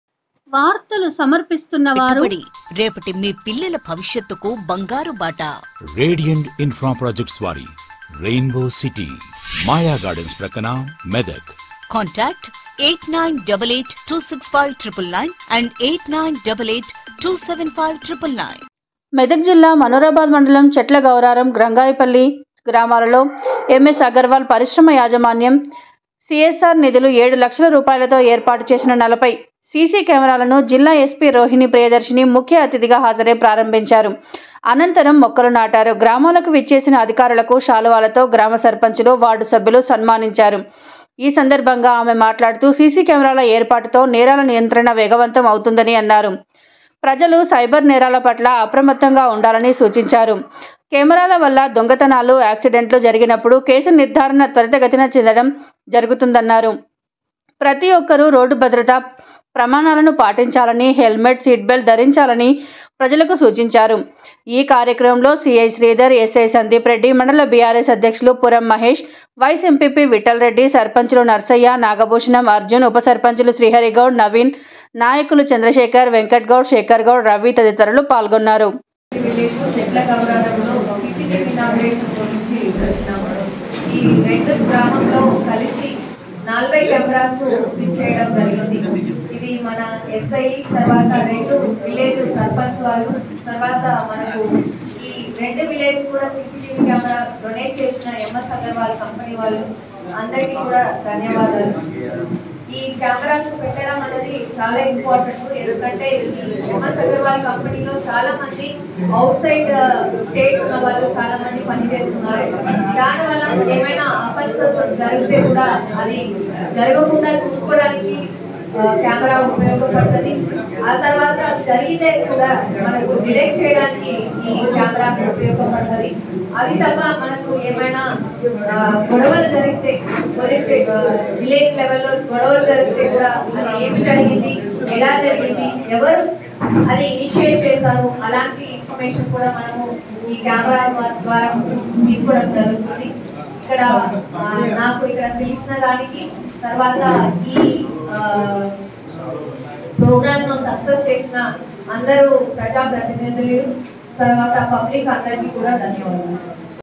మనోహరాబాద్ మండలంలో జిల్లా ఎస్పీ రోహిణి ప్రియదర్శిని సీసీ కెమెరాలను ప్రారంభించారు
మెదక్ జిల్లా మనోహరాబాద్ మండలం చెట్ల గౌరారం, రంగాయిపల్లి గ్రామాలలో
ఎంఎస్ అగర్వాల్ పరిశ్రమ యాజమాన్యం సిఎస్ఆర్ నిధులు ఏడు లక్షల
రూపాయలతో ఏర్పాటు చేసిన 40 సీసీ కెమెరాలను జిల్లా ఎస్పీ రోహిణి ప్రియదర్శిని ముఖ్య అతిథిగా హాజరై  ప్రారంభించారు